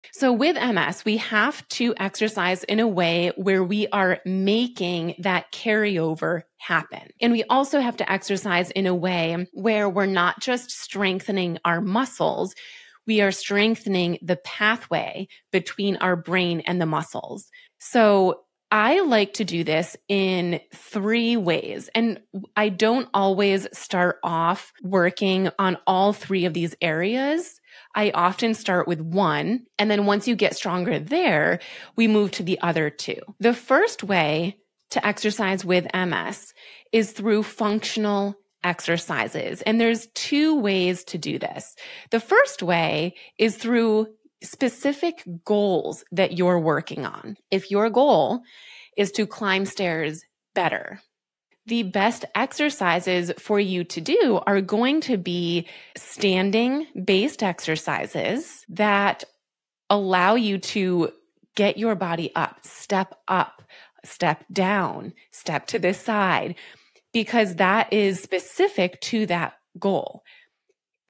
Why Physical Therapy Doesn't Work for MS (And What Does)
In this episode, I’m sharing the real reasons why traditional physical therapy often doesn’t work for people with multiple sclerosis (MS), and—most importantly—what actually DOES help improve strength, mobility, and daily activities for those of us living with MS.
As a physical therapist who specializes in MS, I break down how MS-specific exercises, functional movement strategies, and neuroplasticity can make a big difference in walking, stair climbing, and overall independence.
Tune in for evidence-based advice, practical tips, and empowering exercises tailored specifically for MS symptoms. Whether you’re newly diagnosed or have been managing MS for years, you’ll leave this episode with MS-friendly exercise solutions to help you strengthen your mind-body connection, improve mobility, and boost your quality of life!